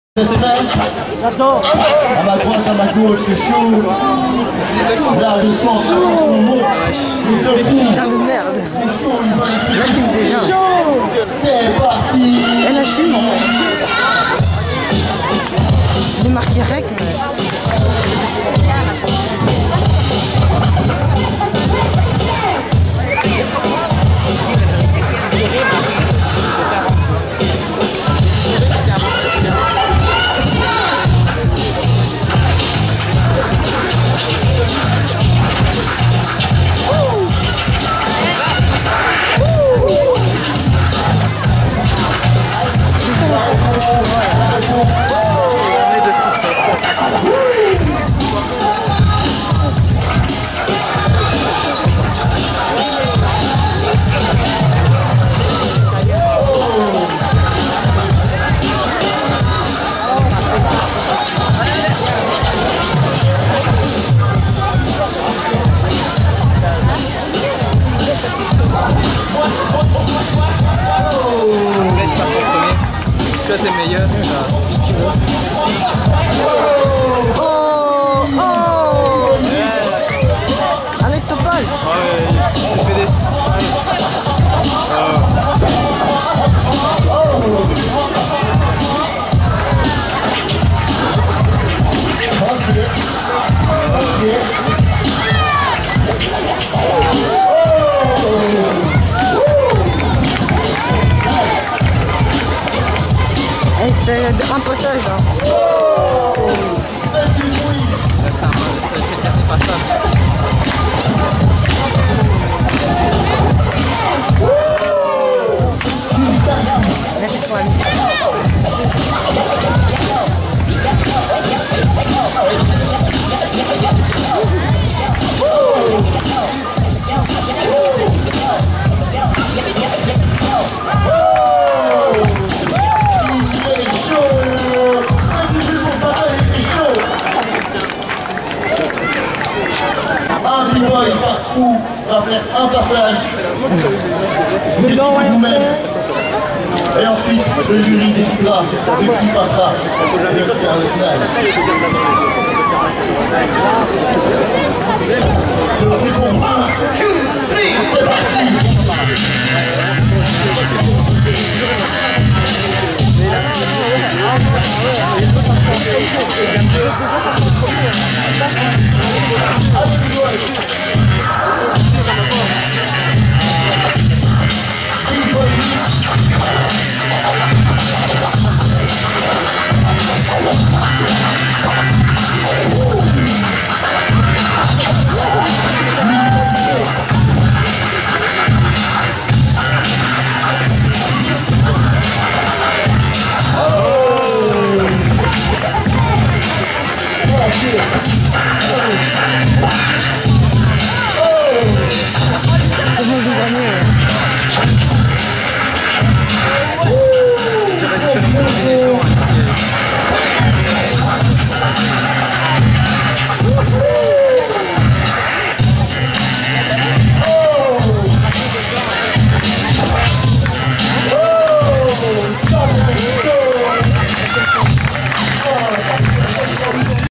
Bboy Topaz & Alpa vs Bena-flex & Corn-flex - Paturage 2011 quart-finale
+ tie break
winner: Bena-flex & Corn-flex